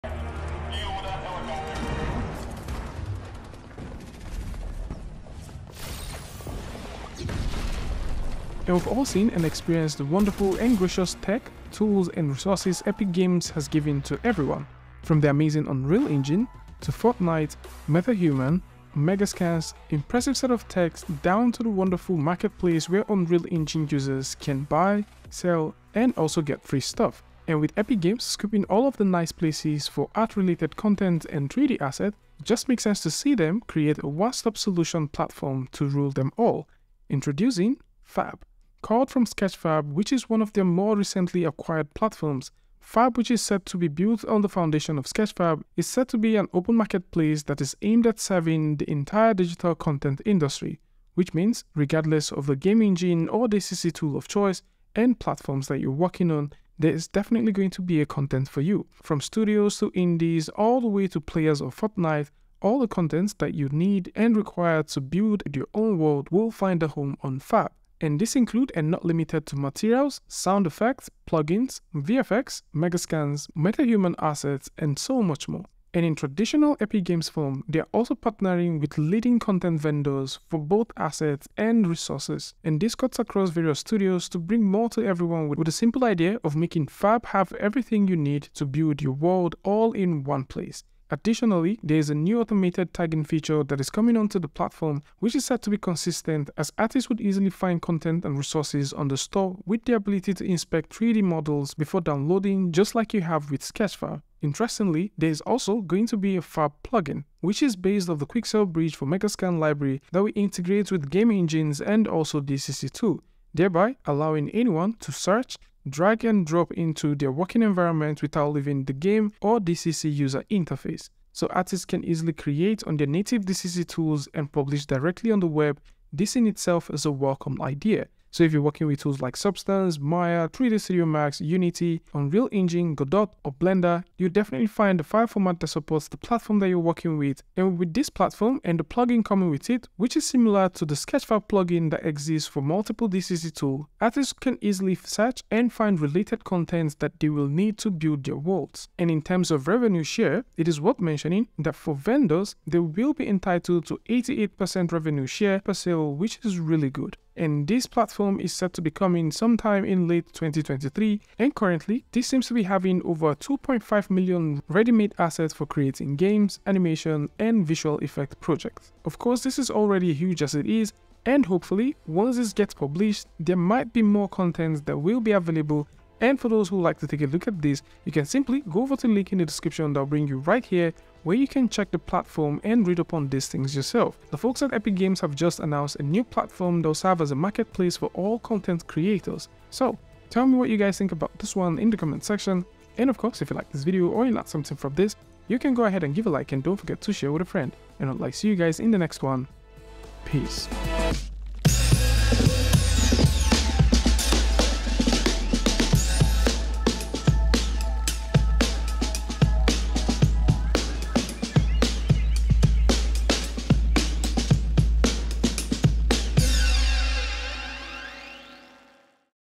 Unreal FAB - A Universal Asset Store For All!
At this point finding assets across platforms can be a chore especially for games and animation across dcc usage.